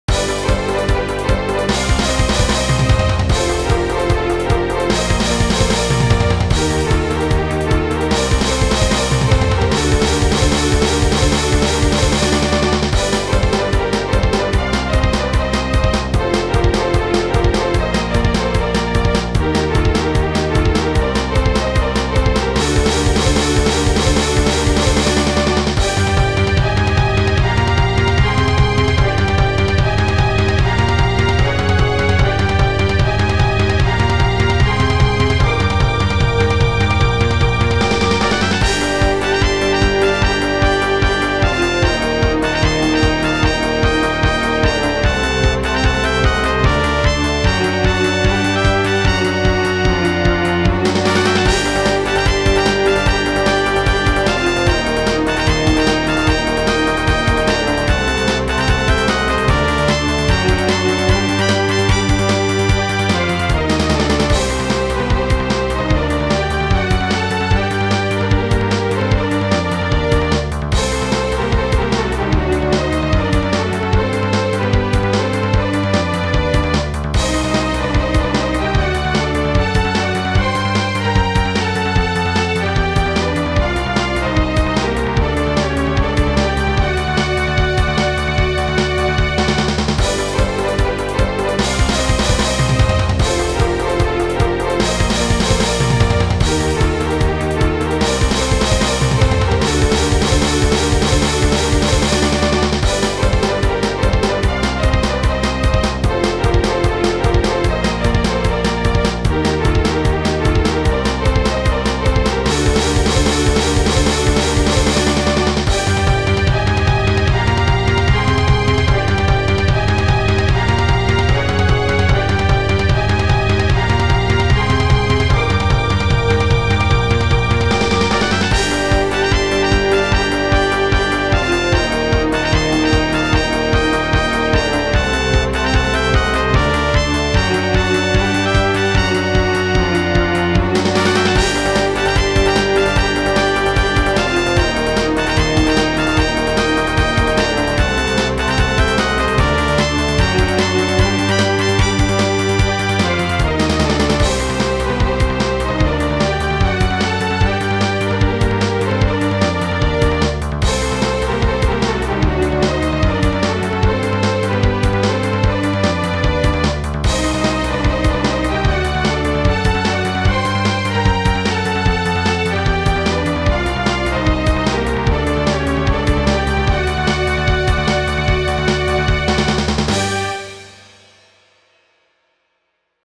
【musicLine】 超大ボス戦 【ロマサガ風】
ロマサガのバトル音楽を目指して作りました。